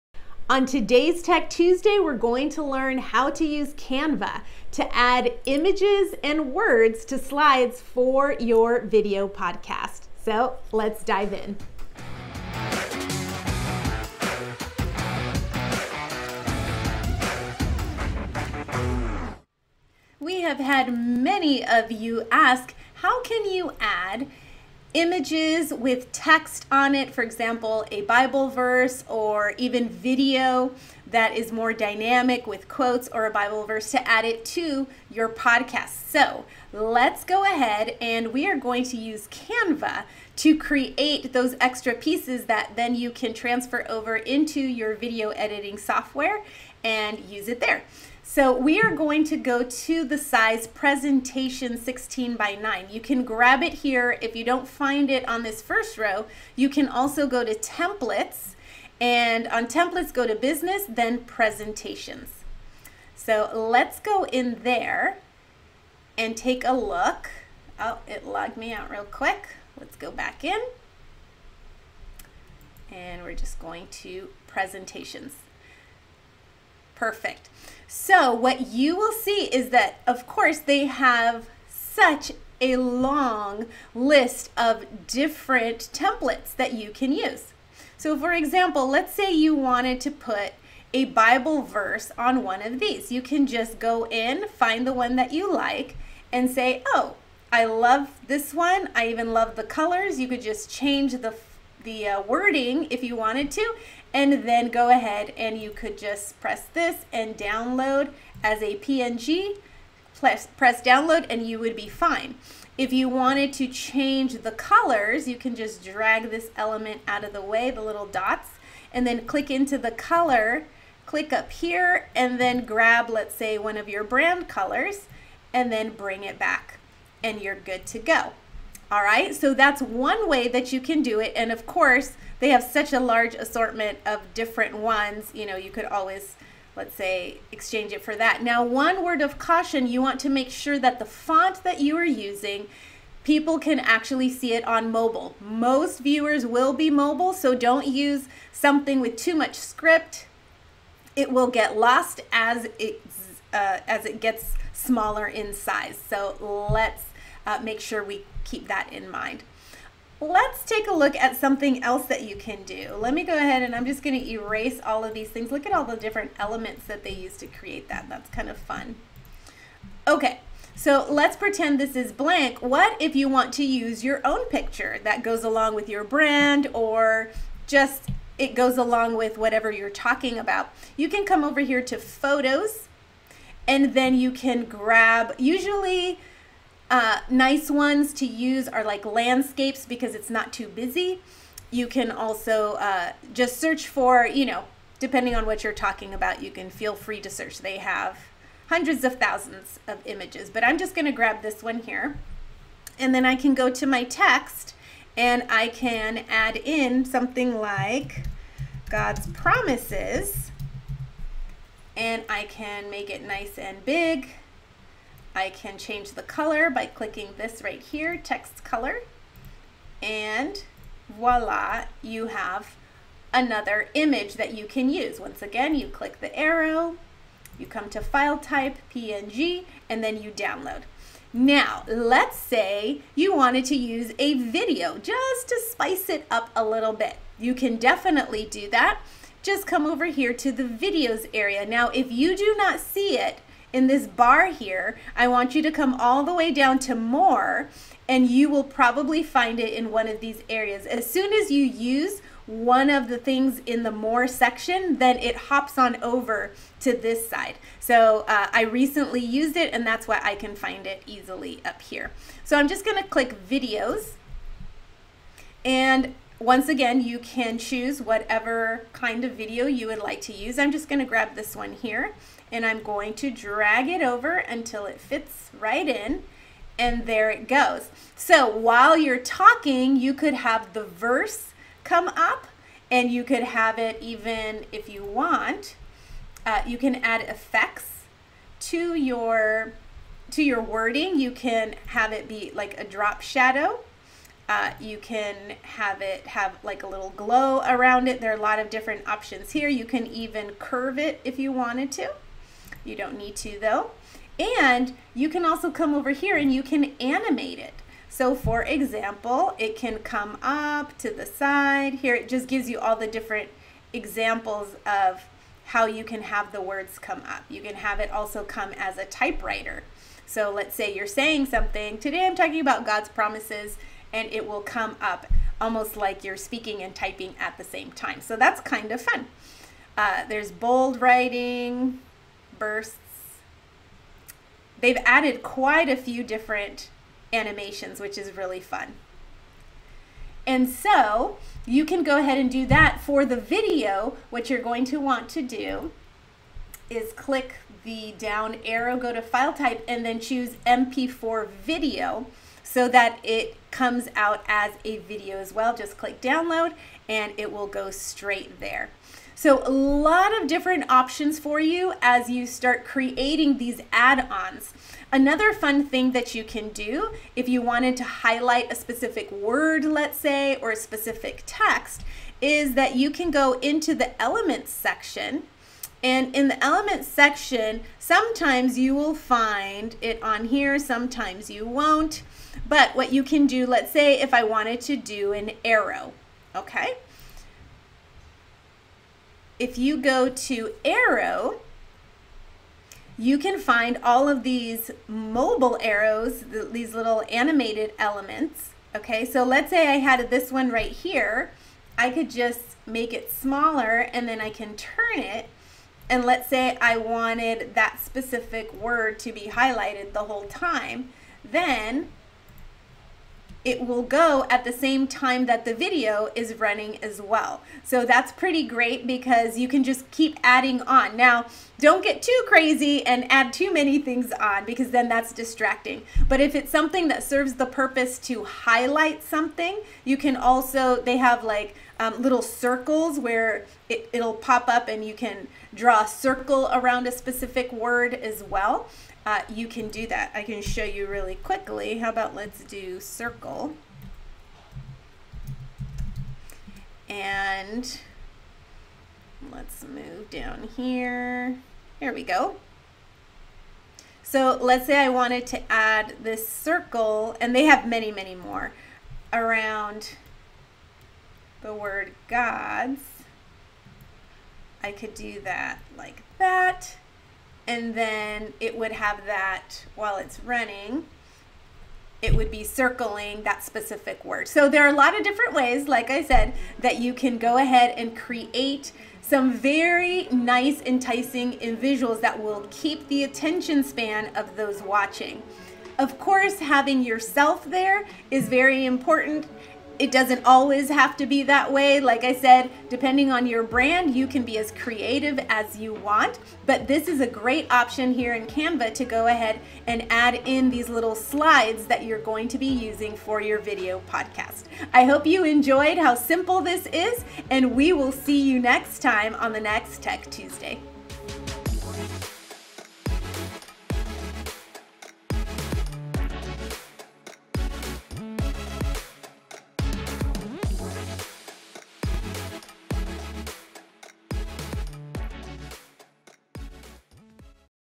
How to add slides to your video podcast using Canva
In this week's Tech Tuesday we will show you how to add slides to your video podcast using Canva.